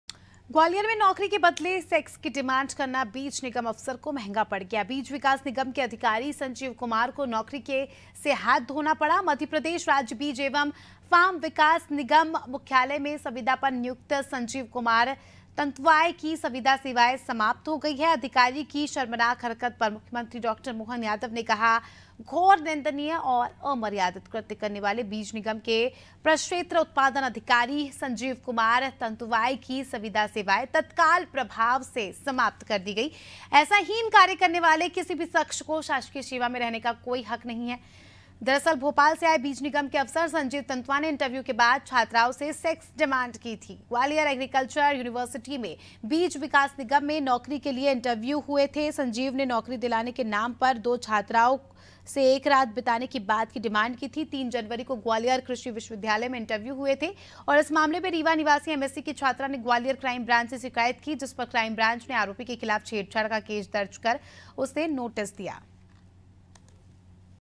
Bhopal : नौकरी के बदले सेक्स की डिमांड, बीज निगम अफसर पर गिरी गाज | MP Hindi News || Anaadi Tv
ग्वालियर में नौकरी के बदले सेक्स की डिमांड करना बीज निगम अफसर को महंगा पड़ गया...बीज विकास निगम के अधिकारी संजीव कुमार को नौकरी से हाथ धोना पड़ा है...मध्य प्रदेश राज्य बीज एवं फार्म विकास निगम मुख्यालय में संविदा पर नियुक्त संजीव कुमार तंतुवाय की संविदा सेवाएं समाप्त कर दी गई है...अधिकारी की शर्मनाक हरकत पर मुख्यमंत्री डॉ. मोहन यादव ने कहा- “घोर निंदनीय और अमर्यादित कृत्य करने वाले बीज निगम के प्रक्षेत्र उत्पादन अधिकारी संजीव कुमार तंतुवाय की संविदा सेवाएं तत्काल प्रभाव से समाप्त कर दी गईं हैं...ऐसा हीन कार्य करने वाले किसी भी शख्स को शासकीय सेवा में रहने का कोई अधिकार नहीं है...दरअसल भोपाल से आए बीज निगम के अफसर संजीव तंतुआ ने इंटरव्यू के बाद छात्राओं से सेक्स डिमांड की थी...ग्वालियर एग्रीकल्चर यूनिवर्सिटी में बीज विकास निगम में नौकरी के लिए इंटरव्यू हुए थे...संजीव ने नौकरी दिलाने के नाम पर दो छात्राओं से एक रात बिताने की डिमांड की थी...3 जनवरी को ग्वालियर कृषि विश्वविद्यालय में इंटरव्यू हुए थे...इस मामले में रीवा निवासी M.Sc. की छात्रा ने ग्वालियर क्राइम ब्रांच से शिकायत की थ...जिस पर क्राइम ब्रांच ने आरोपी के खिलाफ छेड़छाड़ का केस दर्ज कर उसे नोटिस जारी किया था...

AIRTEL - 379